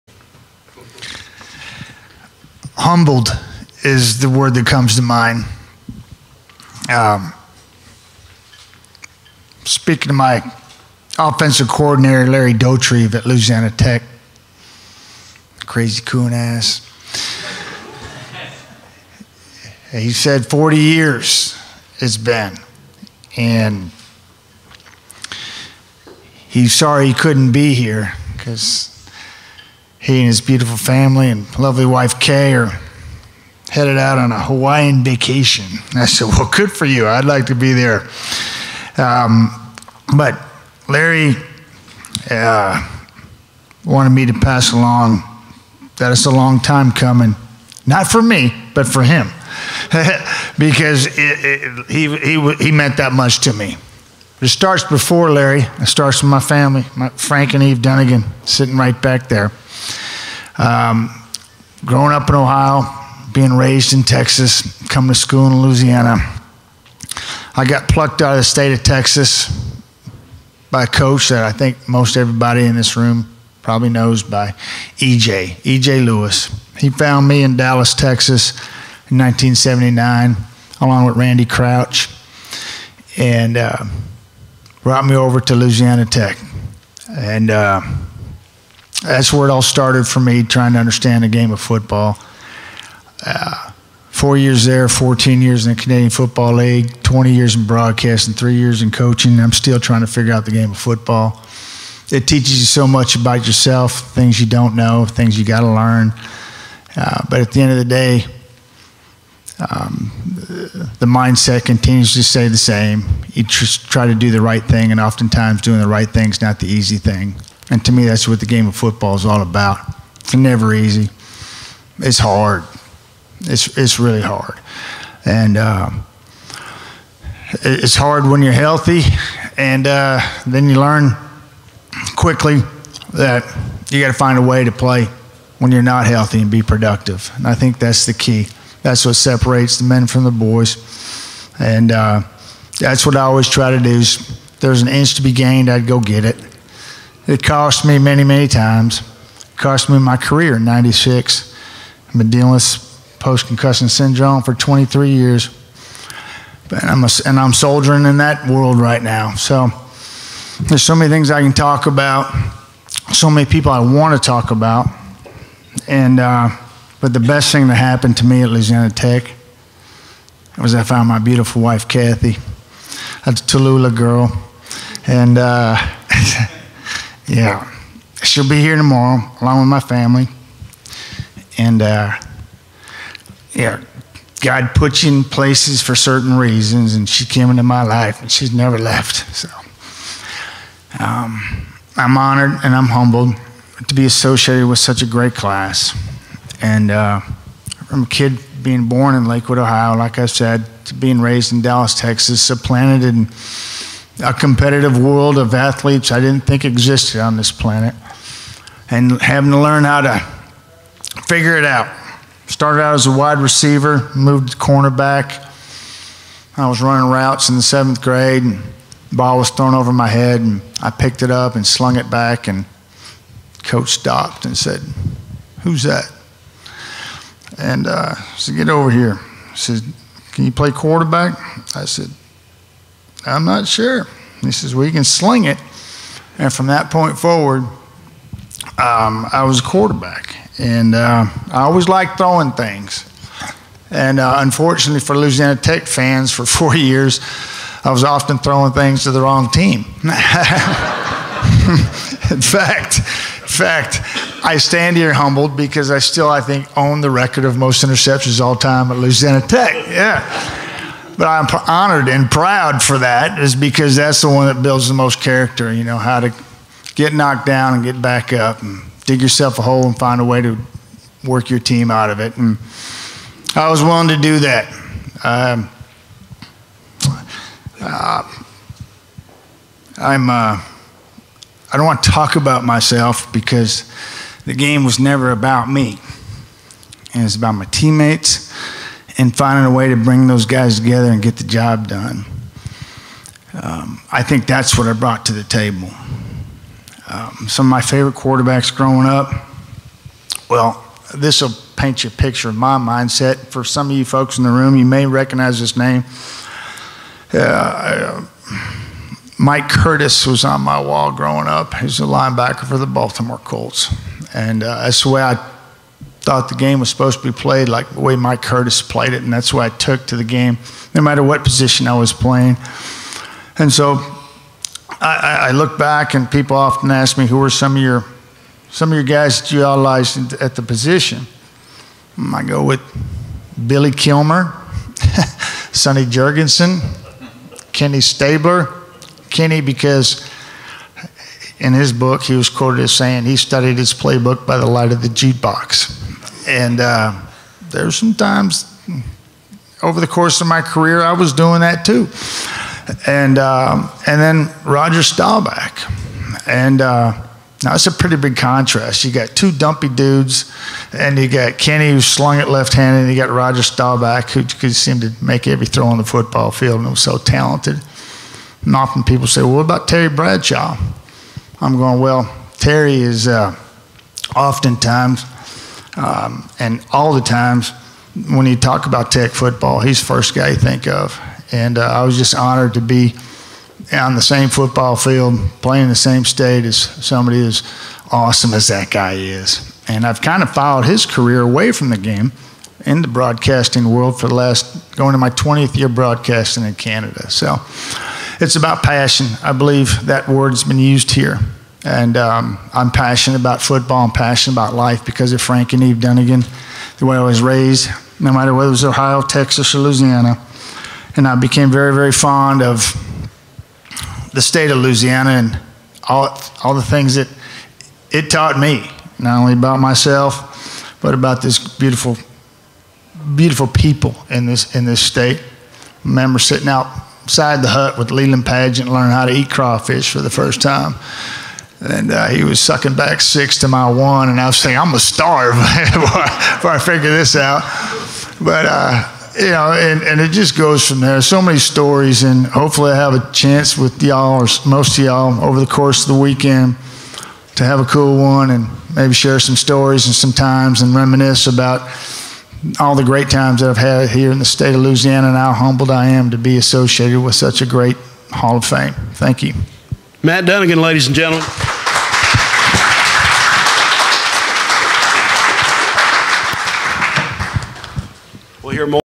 Matt Dunigan - LSHOF News Conference - Thursday, June 6
The 2019 induction class of the Louisiana Sports Hall of Fame speak at a news conference inside the Louisiana Sports Hall of Fame and Northwest Louisiana History Museum in Natchitoches, Louisiana, Thursday, June 6.